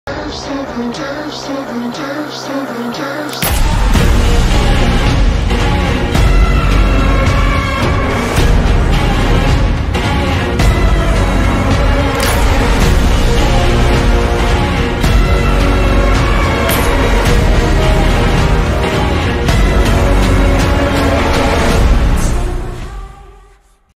''toxicity'' my new 2021 intro!
uhh I haven't actually had an intro for a while, but I kind of want to start making some normal dm videos here and there so I made one for myself (I didn't want to ask anyone since I am not sure how much I'll use it haha)

credits to irreplaceable dm for the mask zoom thingy idea (fast zooms with a mask on top), I am pretty sure she did it first and it's so creative ahh just go check ana out, she doesn't have nearly enough subs!!